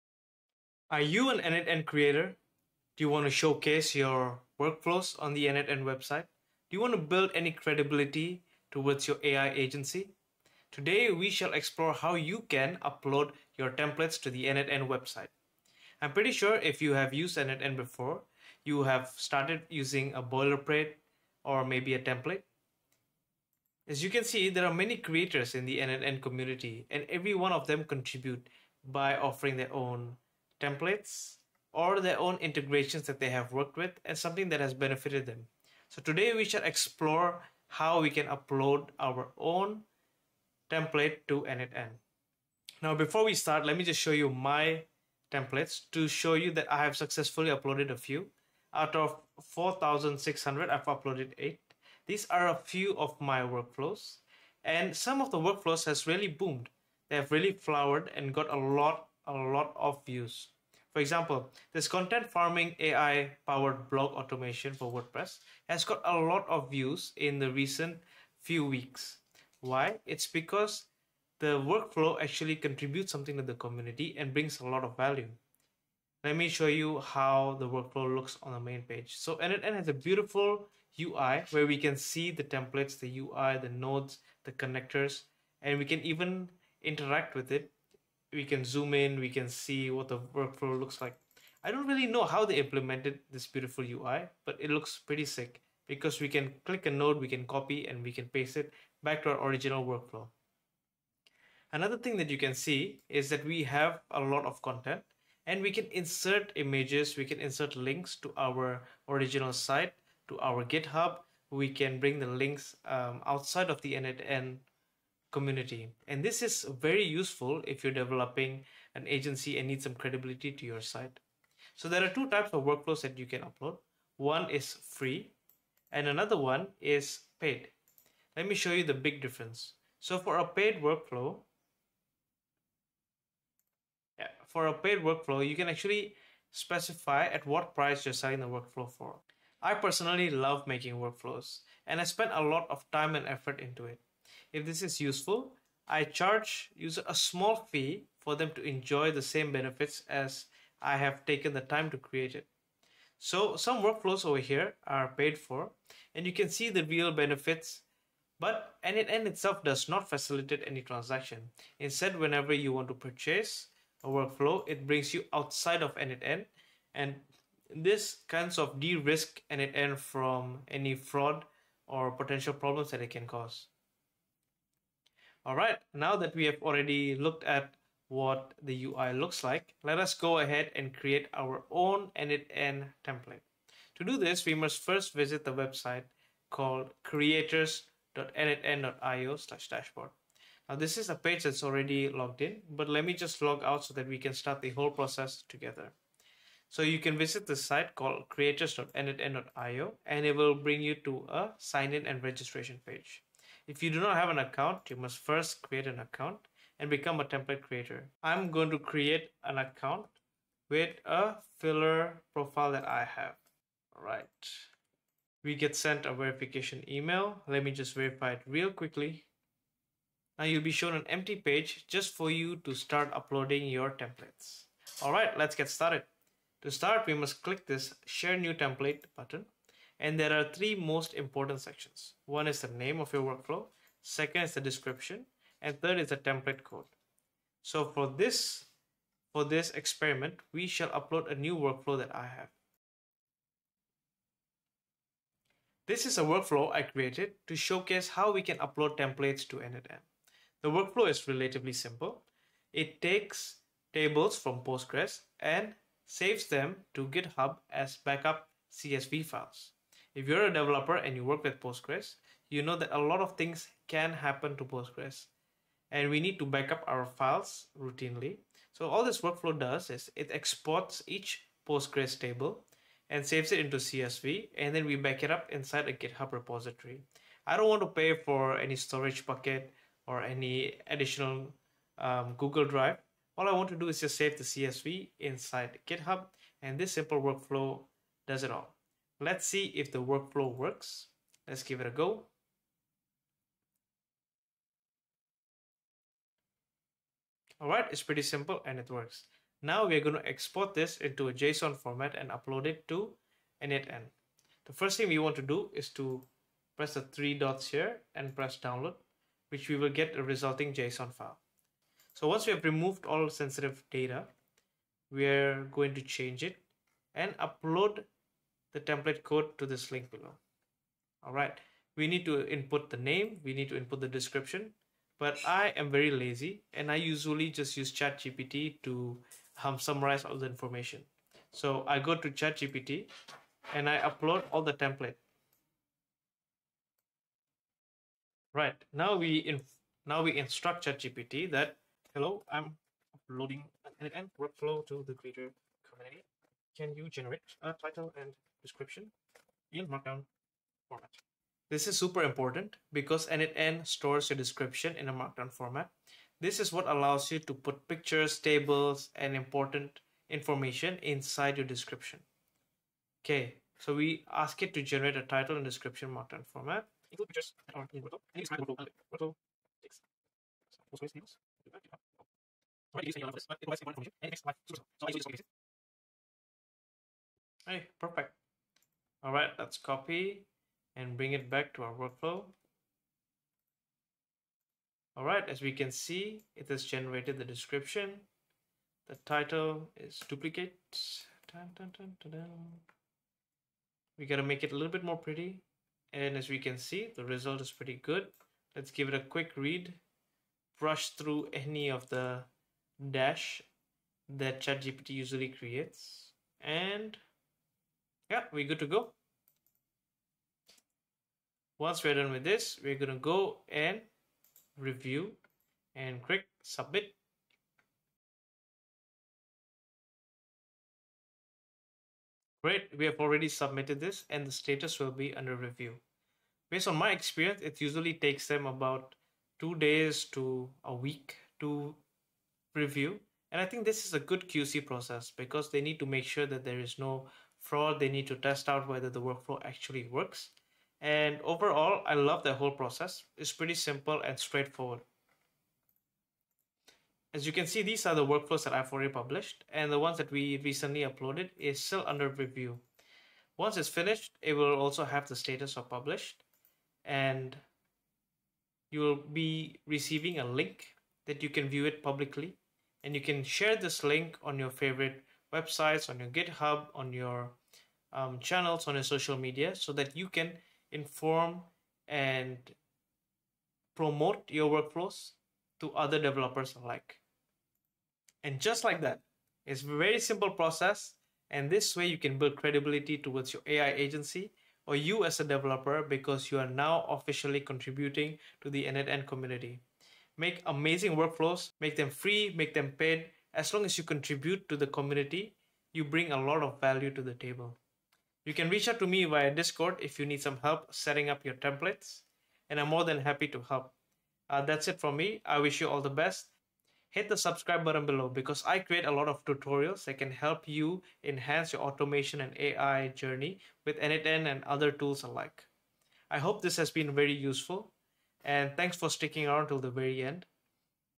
Publish Your n8n Workflow & Reach 10,000 Creators Fast
Want your n8n workflows to get discovered, cloned, and used by thousands of other creators?
In this video, I’ll show you exactly how to publish your n8n workflow in the official n8n Creators Community step by step

You’ll learn:
✅ How to register for an n8n creator account
✅ How to prepare your workflow for public sharing
✅ How to write a title & description that attracts downloads
✅ Create a markdown description for your workflows
✅ How to submit and get your template approved

TIMESTAMPS:
00:00 Introduction
00:45 Demo
02:01 Paid vs Free Workflows
03:07 Register as n8n Creator
03:48 Upload your first workflow
05:33 Use ChatGpt to create workflow description
06:58 Submit your workflow
07:58 Like + Subscribe :)